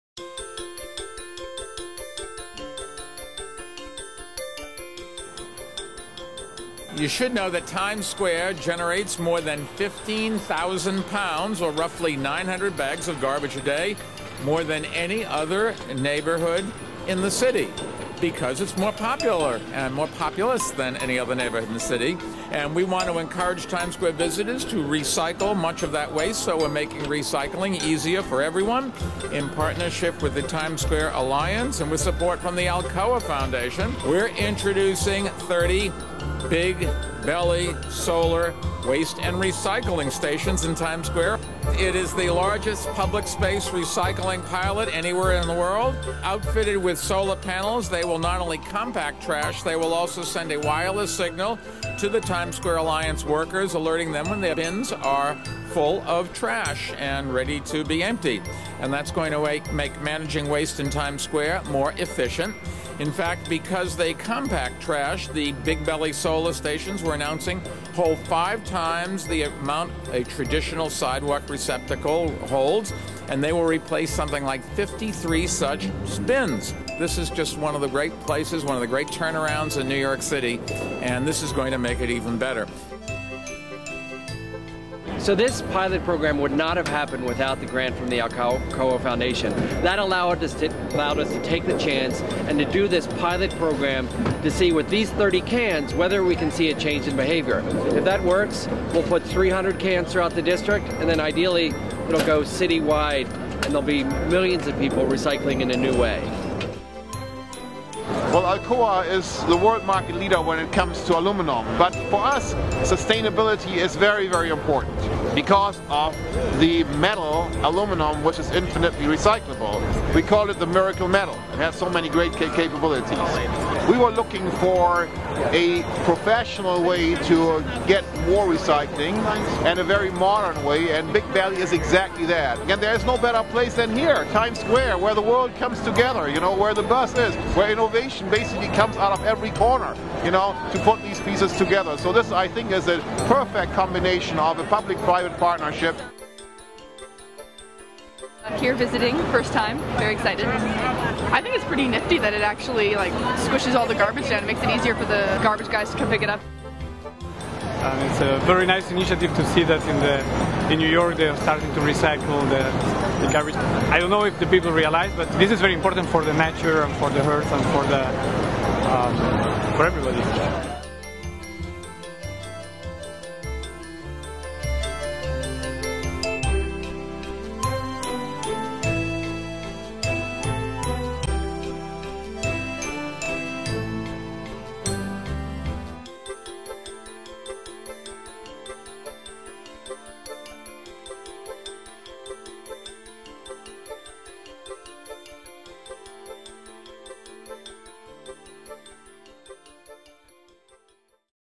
Times Square Goes Green with Alcoa, BigBelly and Mayor Bloomberg
March 15, 2013 — New York, NY — Mayor Michael R. Bloomberg and the Times Square Alliance, in partnership with the Alcoa Foundation and BigBelly Solar, announced the debut of the largest public space recycling initiative in New York City. The pilot, which will bring 30 new BigBelly solar-powered waste and recycling stations to Times Square, will encourage public space recycling among the more than 500,000 daily visitors to the crossroads of the world. The Alcoa Foundation granted Times Square Alliance the opportunity to install the 30 Big Belly units that will be placed throughout Times Square. The Mayor was joined at the announcement in Times Square by Sanitation Commissioner John J. Doherty, Alcoa Chairman and CEO Klaus Kleinfeld, Times Square Alliance President Tim Tompkins and BigBelly CEO Jim Norrod.